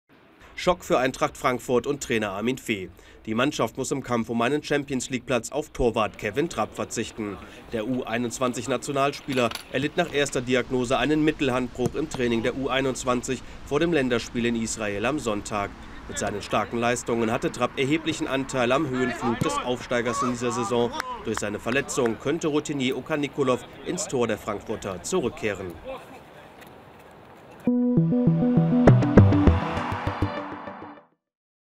Schock für Frankfurt: Trapp erleidet Mittelhandbruch
Fußball-Bundesligist Eintracht Frankfurt muss für längere Zeit auf Stammtorwart Kevin Trapp verzichten. Der 22-Jährige brach sich im Training der U21-Nationalmannschaft die Hand.